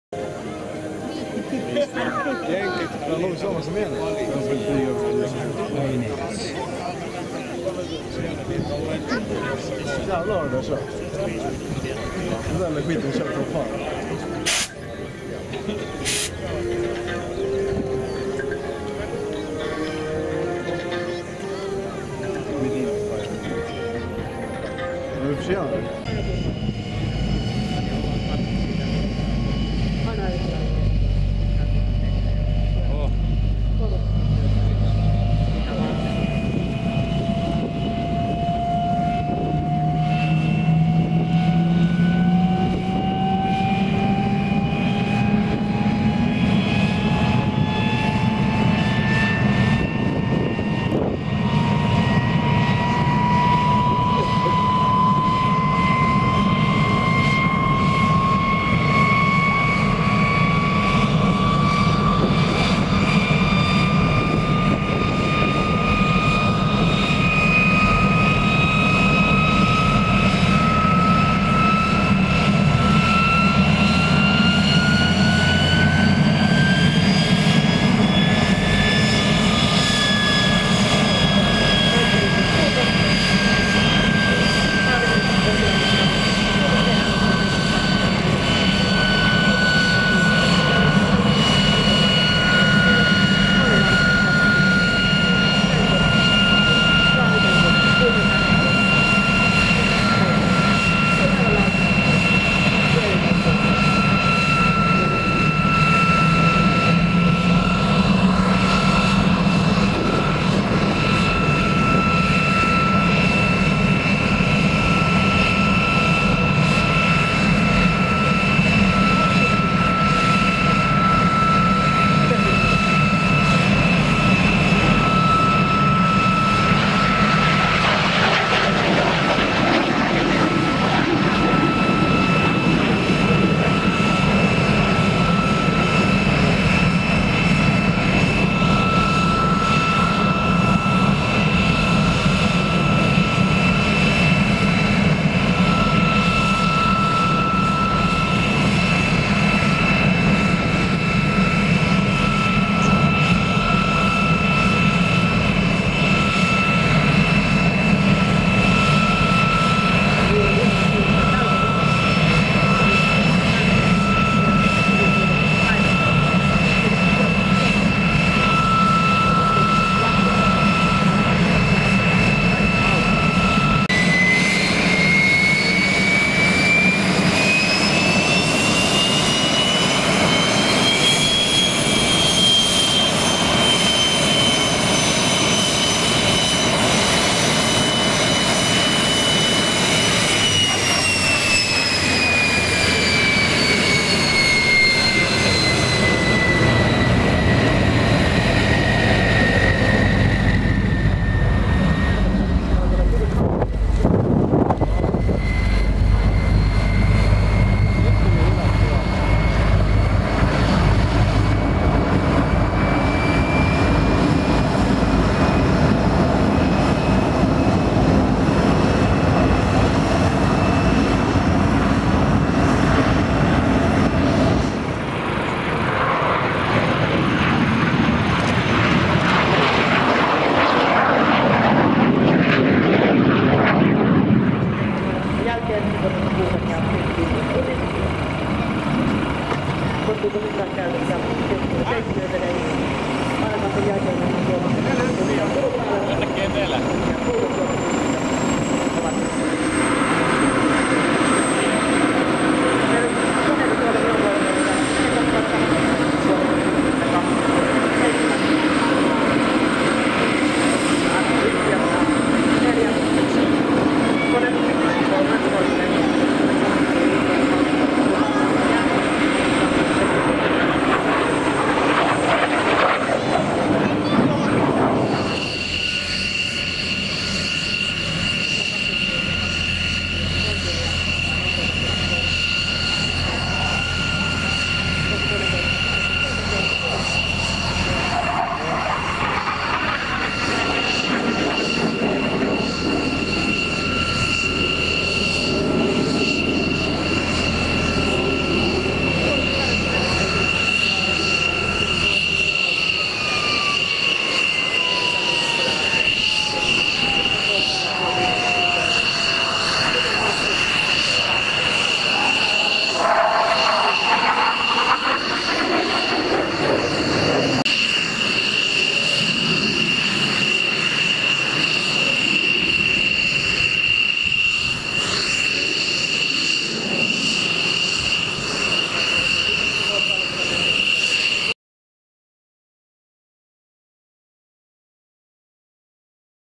Mig-15 UTI in Turku airshow
Royal Norwegian historic squadron at Turku Airshow 6.6.2015. Norwegian MiG-15 UTI was painted in Finnish air force markings for display. Total 4 of MiG-15 UTI was used in Rissala Kuopio during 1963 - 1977.  There was minor landing gear problems after flight as you can see.